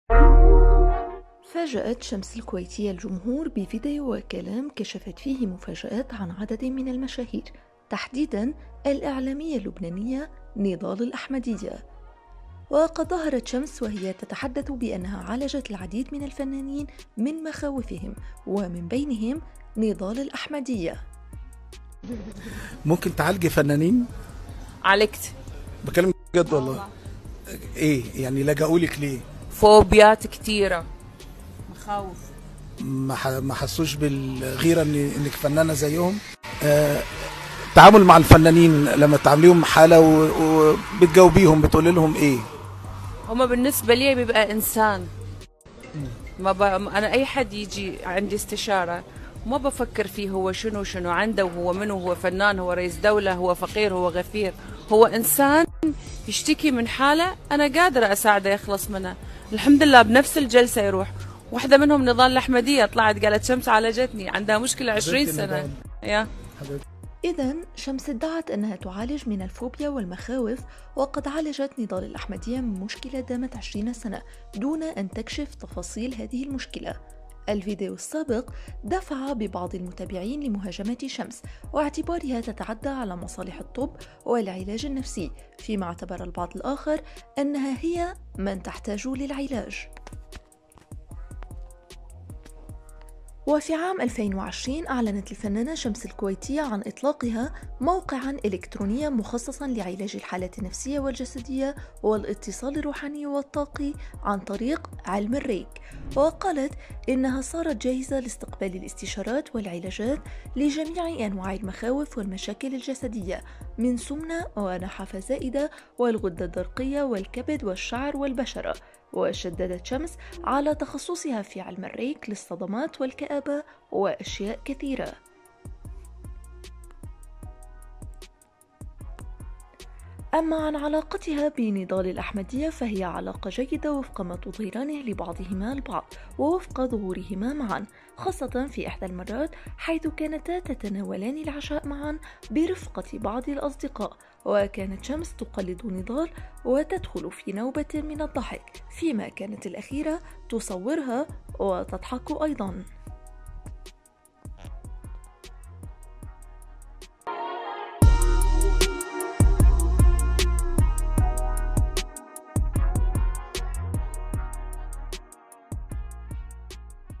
شمس الكويتية تكشف مر ض نضال الاحمدية وتقول : عالجتها من مر ض خـ.ـطير
شمس الكويتية تكشف مر ض نضال الاحمدية وتقول : عالجتها من مر ض خ_طير